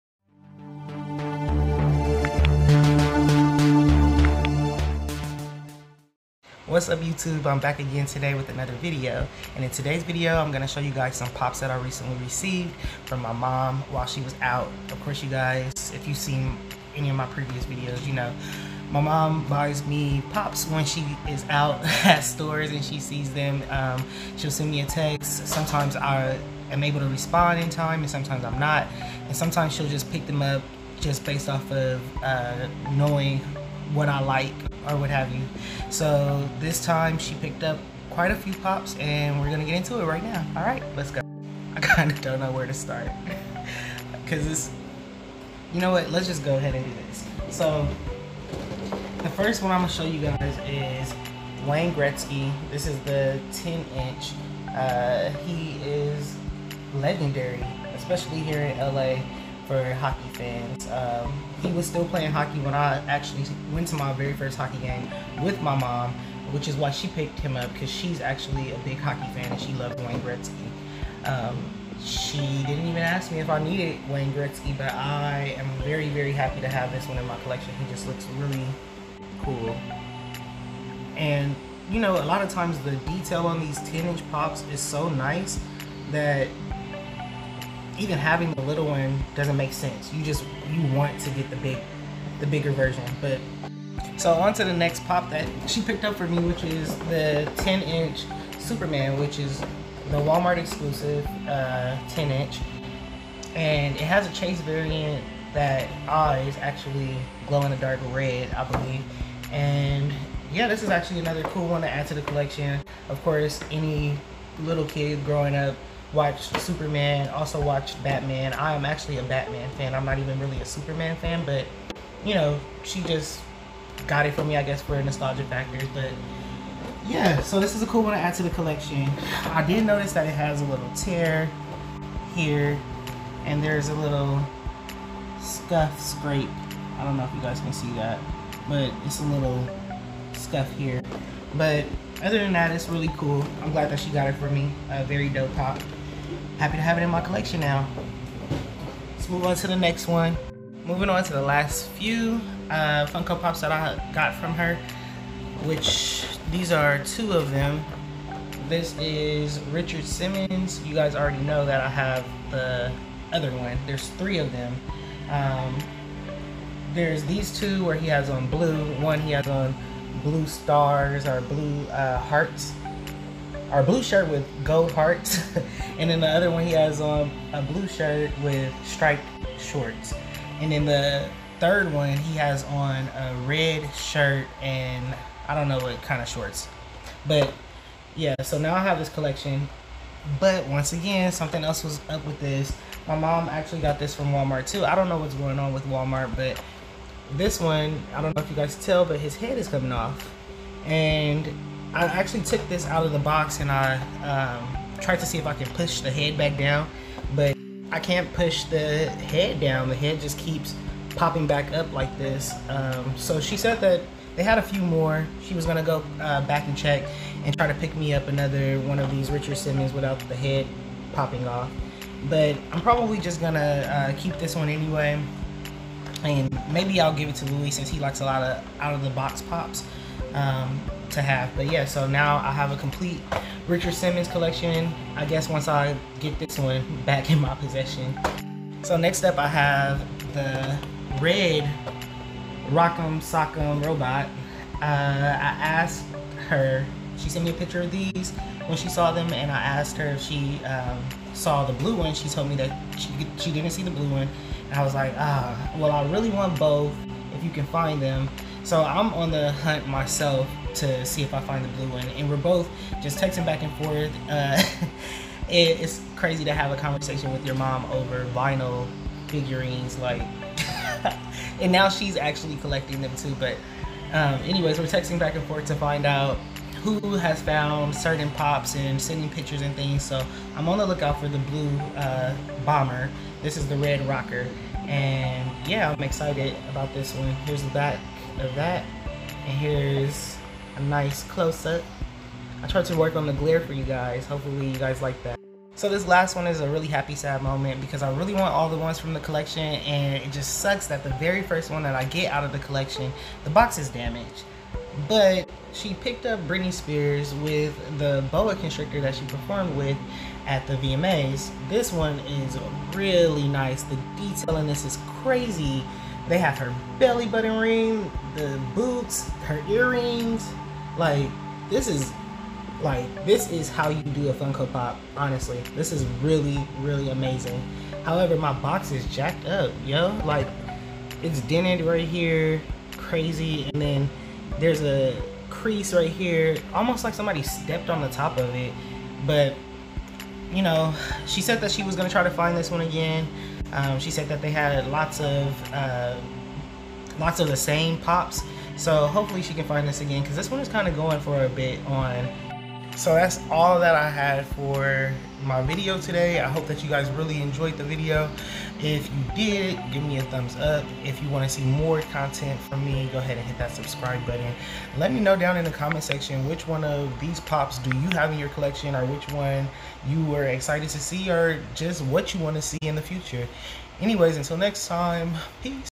More Walmart Funko Pop Finds
In this video i show you some Pops my mom grabbed for me while she was out shopping.

Tell me what you guys think let me know what Funko Pops you guys are waiting on and anything Pop related you would like to see in the future.